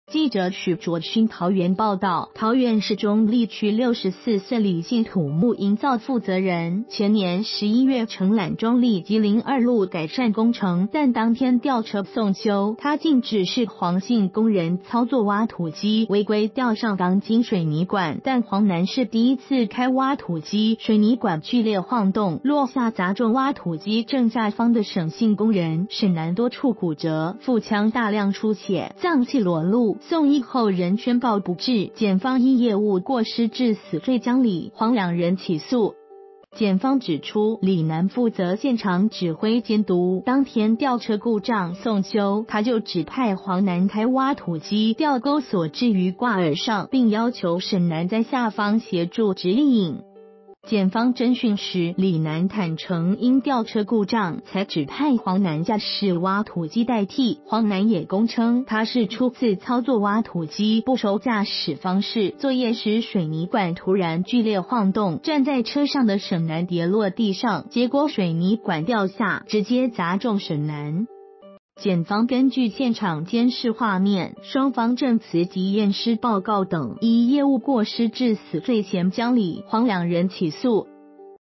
挖土機當吊車用 害工人被水泥管砸死
〔記者許倬勛／桃園報導〕桃園市中壢區64歲李姓土木營造負責人，前年11月承攬中壢吉林二路改善工程，但當天吊車送修，他竟指示黃姓工人操作挖土機，違規吊上鋼筋水泥管，但黃男是第一次開挖土機，水泥管劇烈晃動，落下砸中挖土機正下方的沈姓工人，沈男多處骨折、腹腔大量出血，臟器裸露，送醫後仍宣告不治，檢方依業務過失致死罪將李、黃2人起訴。

發表評論評論如下。